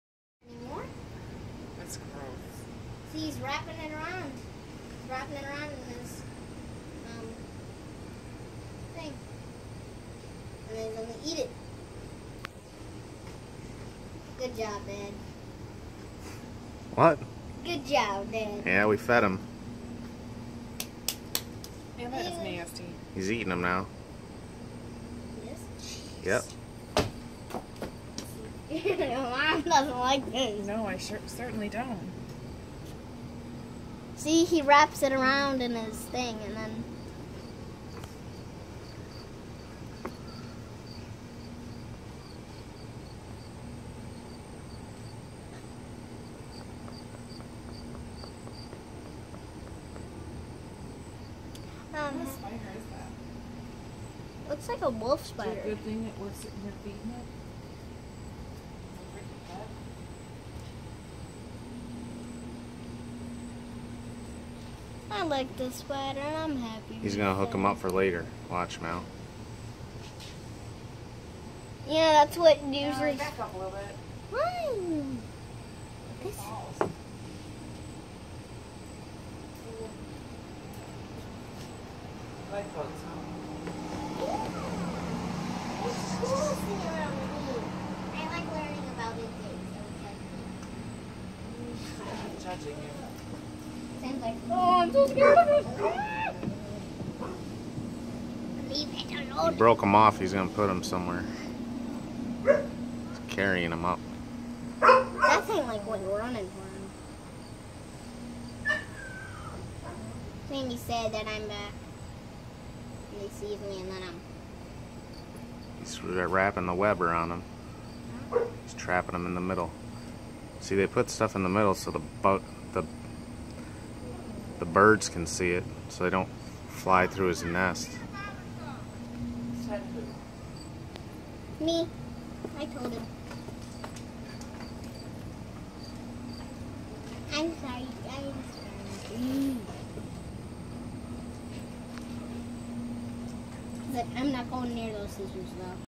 Garden Orb Weaver Spider vs. Earwig......wrap job and transport.
LaSalle, IL August 2016. these get bigger as fall aproaches. I will keep him going all fall.